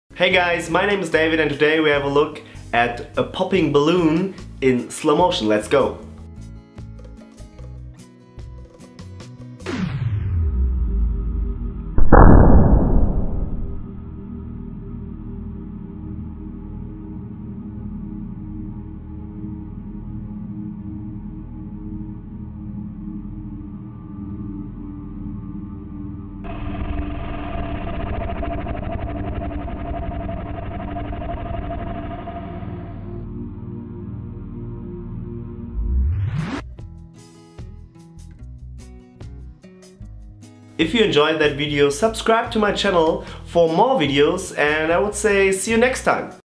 Balloon pop in Slow Mo | 2500 fps | Eye Candy Slow Motion
If a balloon pops it is always very fast. David wanted to know what happens when a balloon pops so he watches it in slow motion. Suprisingly you can see how the air condensates when the balloon pops.